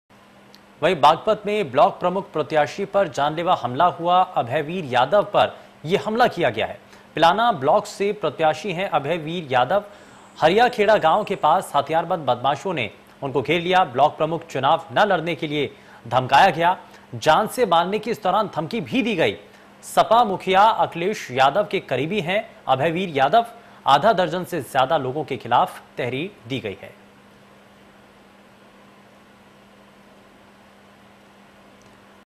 यूपी, बागपतः ब्लॉक प्रमुख प्रत्याशी अभयवीर यादव पर जानलेवा हमला, चुनाव न लड़ने की मिली धमकी
यूपी, बागपतः ब्लॉक प्रमुख प्रत्याशी अभयवीर यादव पर जानलेवा हमला, चुनाव न लड़ने की मिली धमकी India News UPUK